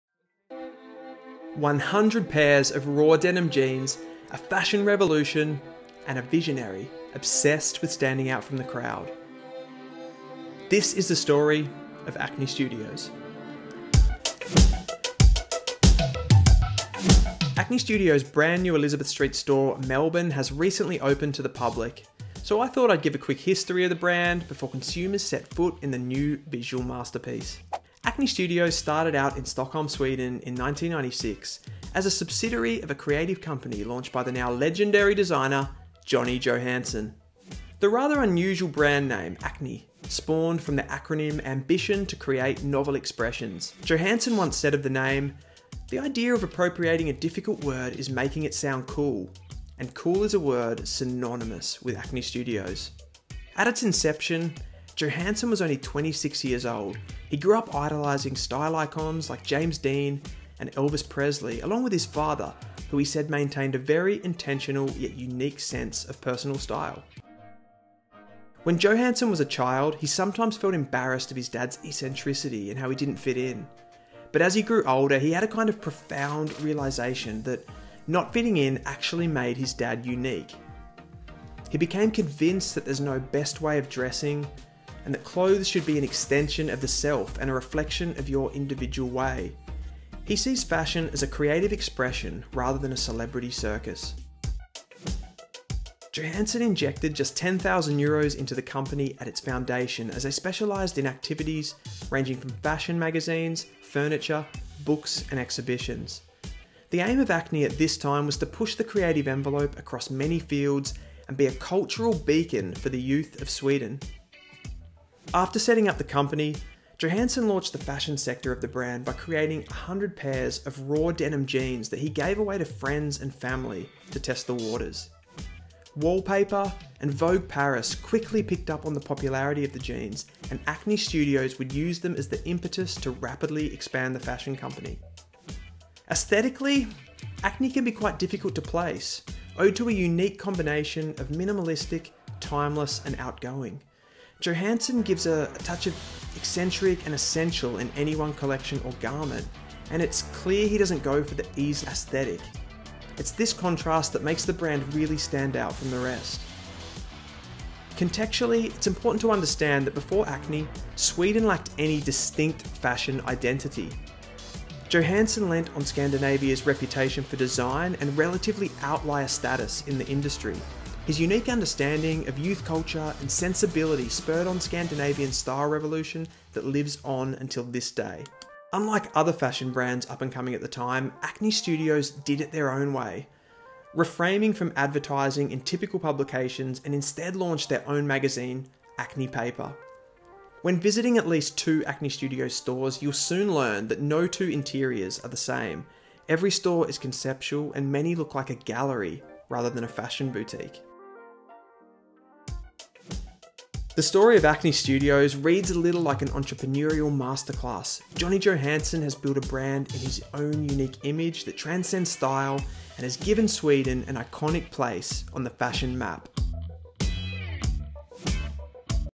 The Story of Acne Studios
Acne studios is a fashion brand that needs no introduction. Popular amongst many demographic groups, Acne is a national treasure in Sweden that has revolutionised fashion. This video is a quick overview of the birth of a generational brand.